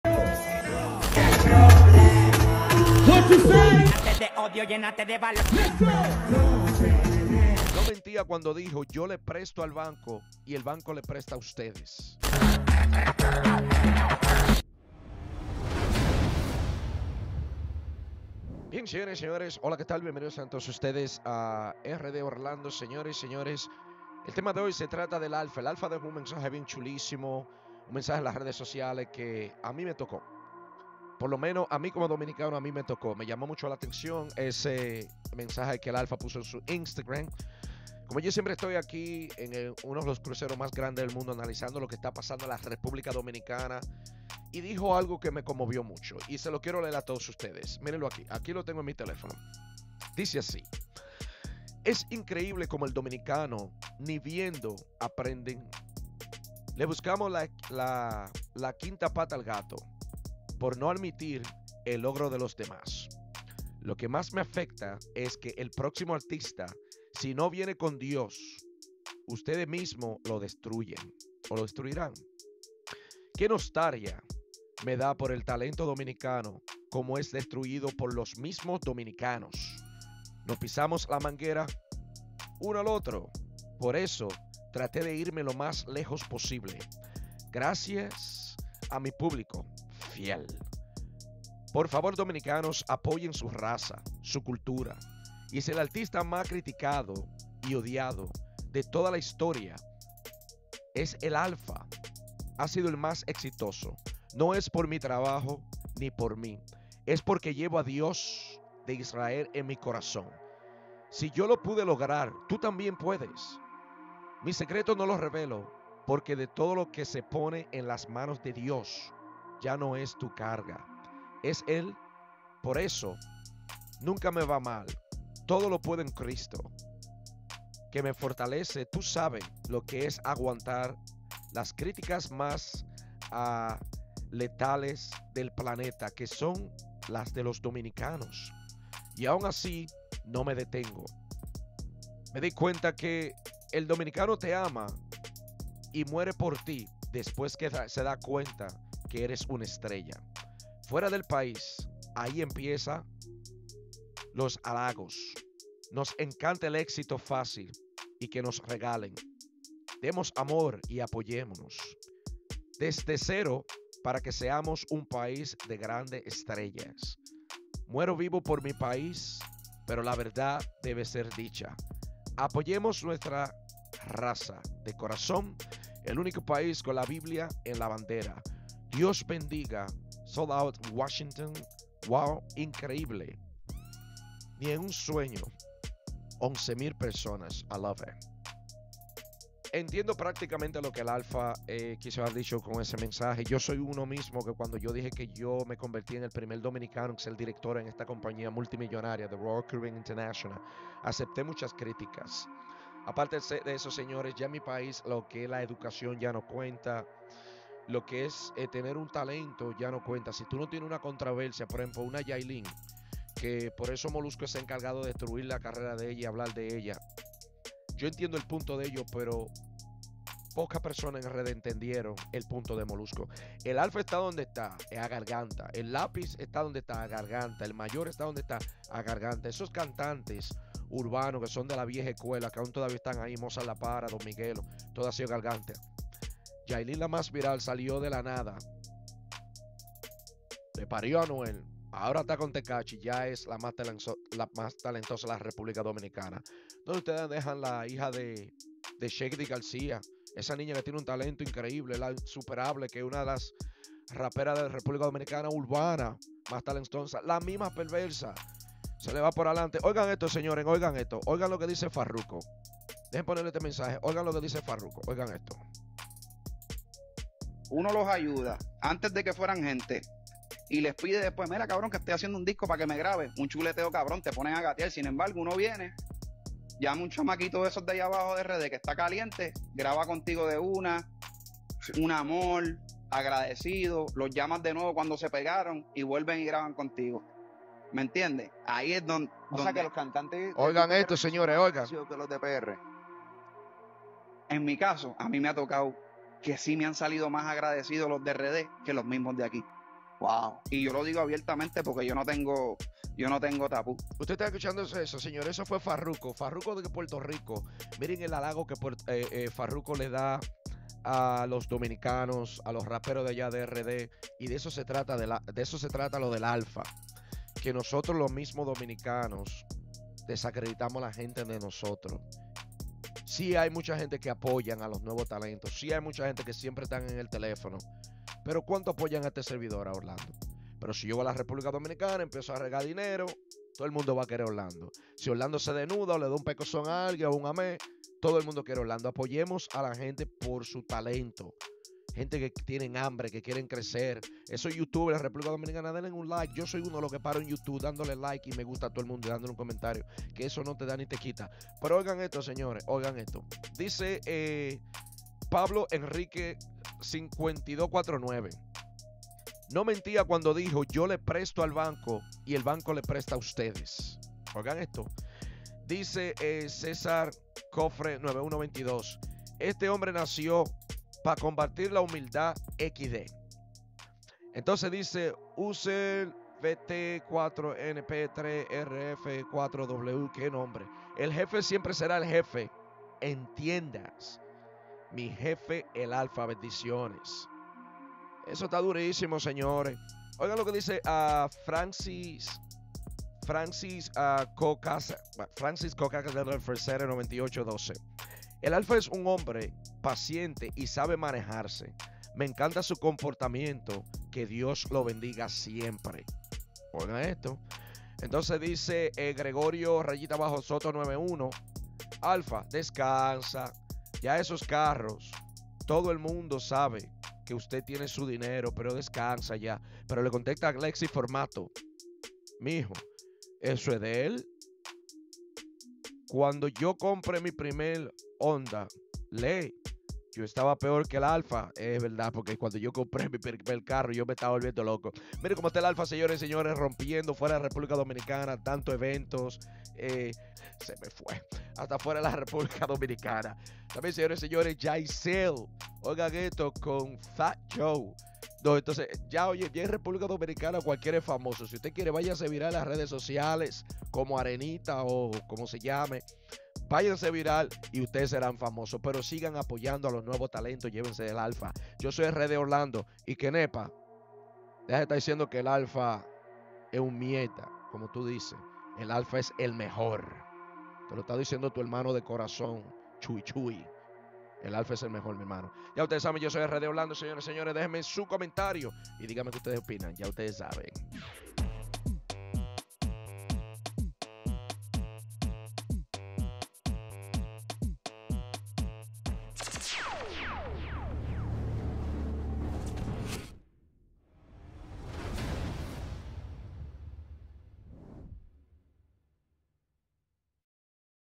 El Alfa El Jefe Habla Sobre Las Críticas de Los Dominicanos
El alfa el jefe

Se desahogo en su instagram que los dominicanos critican a sus propios paisanos, a los dominicanos le molesta el crecimiento de los demás, creo que no todos son iguales en República Dominicana.

El alfa el jefe Dios lo ha bendecido mucho en su carrera musical y sus logros hansido gracias a Dios y sus esfuerzos.

todos sabemos que a los dominicanos le gusta la controversia y las competencias entre ellos, el alfa siempre será el Rey del dembow dominicano.